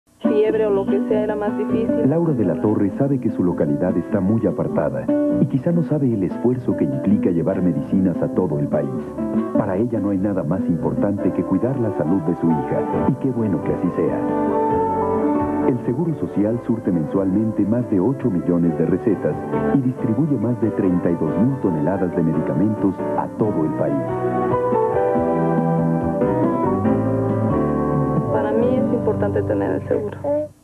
Comerciales mexicanos: IMSS 1996 2
Llegada de toneladas de medicamentos a todo el pais